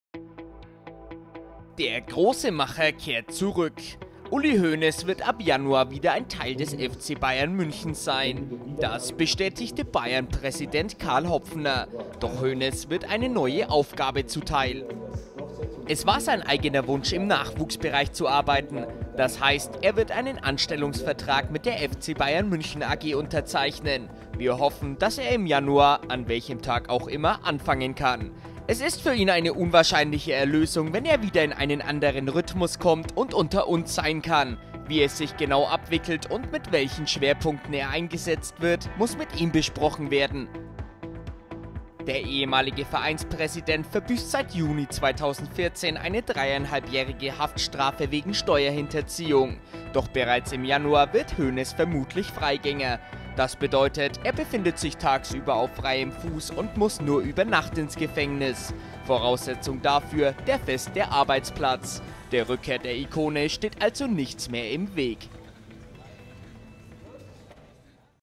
Uli Hoeneß im Januar zurück beim FC Bayern München | Karl Hopfner bestätigt Rückkehr zum FCB
Nur sieben Monate nach seinem Haftantritt wird Uli Hoeneß zum FC Bayern München zurückkehren. Bayern-Präsident Karl Hopfner bestätigte ein geplantes Comeback im Januar.